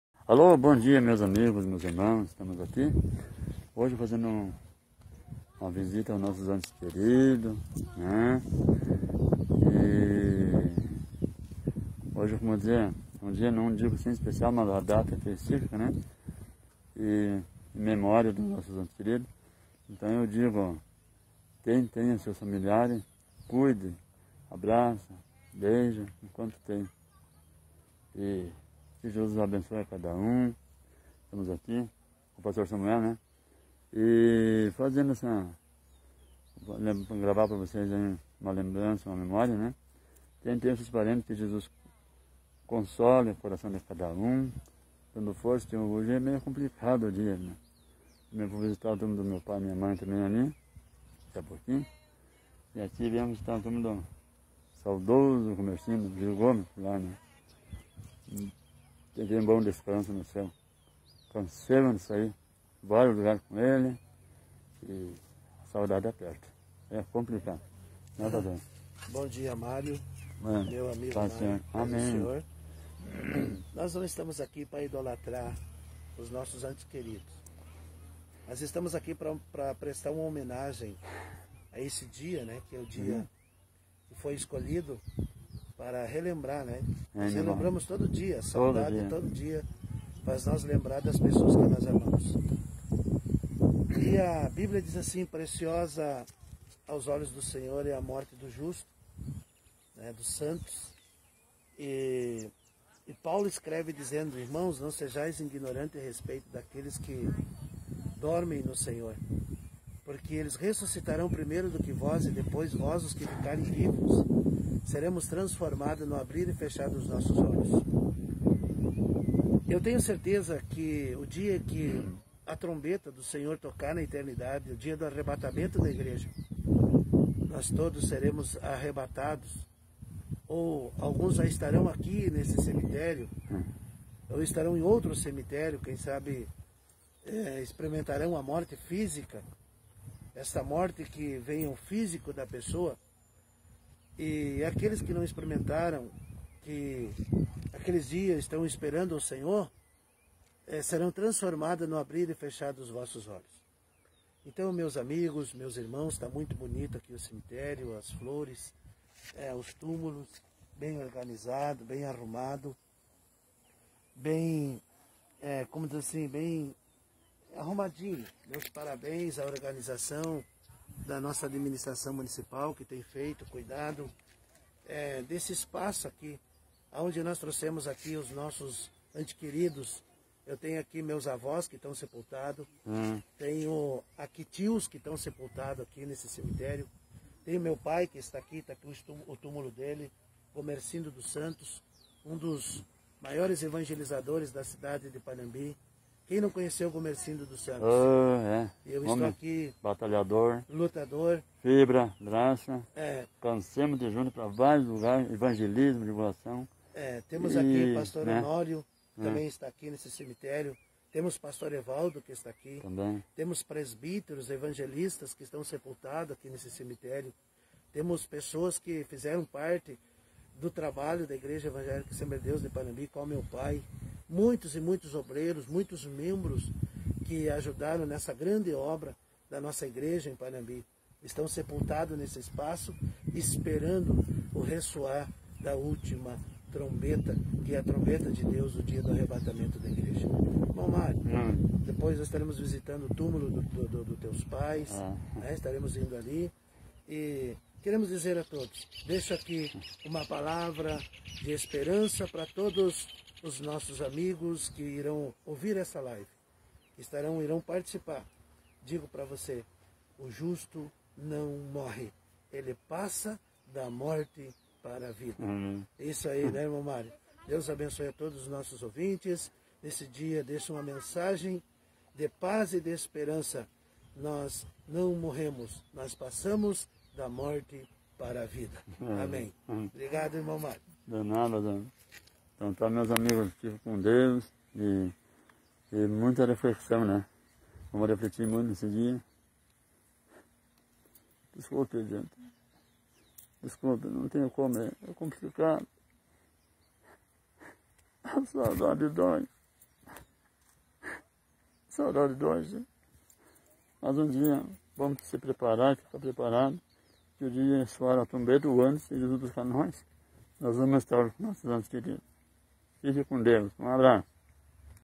Dos antes querido Mario pastor Samuel Sanfoneiro ouro e prata
saudades memorial Dos antes querido Mario pastor Samuel Sanfoneiro ouro e prata visitando os túmulo dos ante queridos já falecido